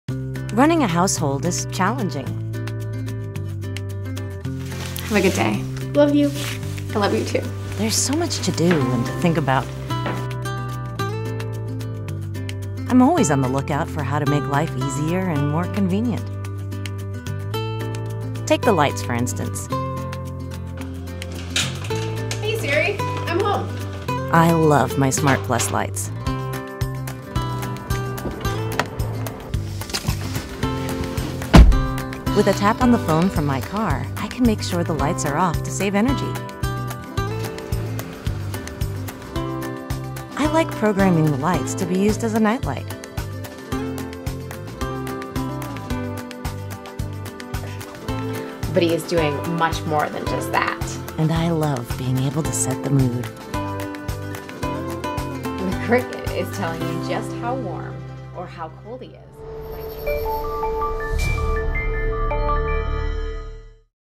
SYLVANIA SMART+ and Apple HomeKit for your Family
SYLVANIA SMART+ is quick and easy to set up with Apple® HomeKit™. See how smart lighting with SYLVANIA SMART+ can work for you and your family.